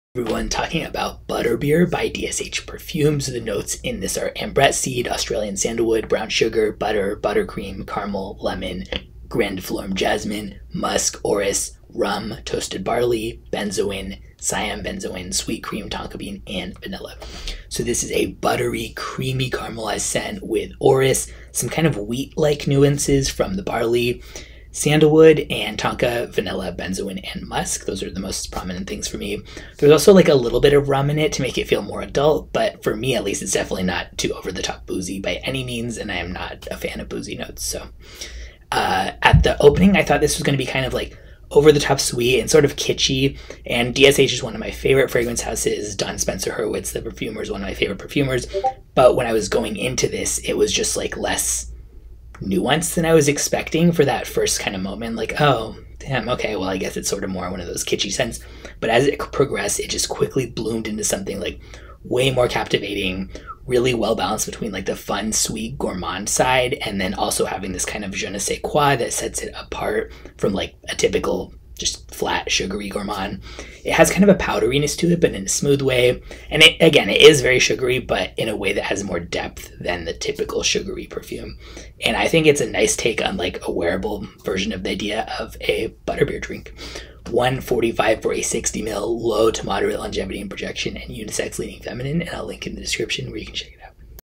DSH Perfumes Butterbeer Review | Creamy Caramelized, Orris, Barley, Sandalwood, Vanilla | Unisex
Review of Butterbeer by DSH Perfumes.

The notes in this fragrance are:

Ambrette Seed, Australian Sandalwood, Brown Sugar, Butter, Buttercream, Caramel, lemon, Grandiflorum Jasmine, Musk, Orris, Rhum, rum, roasted barley, barley, benzoin, siam benzoin, sweet cream, tonka bean, vanilla